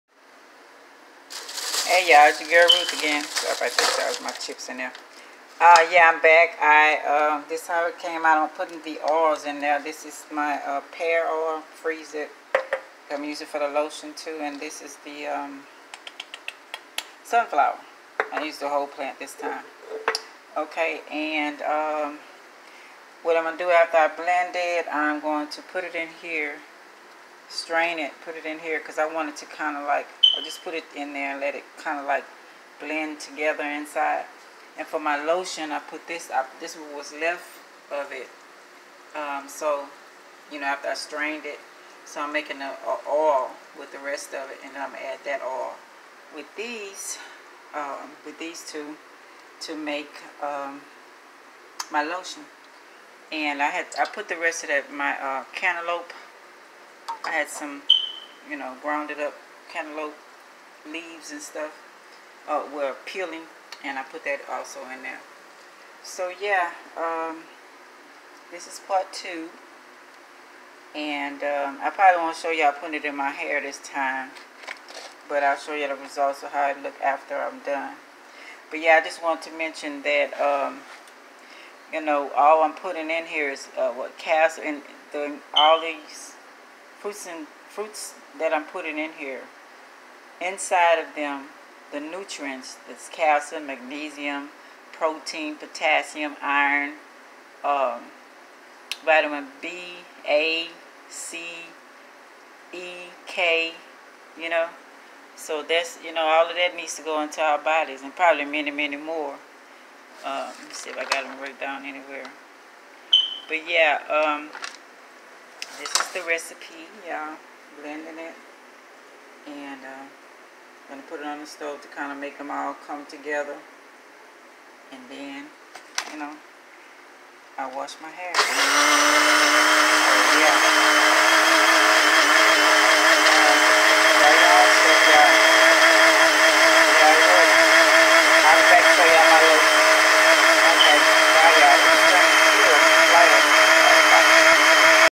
P2 how to make 2Brain Shampoo with canalope and more
Adding oils to shampoo, and starting lotion with same ingredients ruthrobinson with cucumbers, onion, strawberries, alvacados, pear oil, sunflower oil, apple cider vingar, cactus, cantalope, beeswax, castor oil, coconut oil olive and more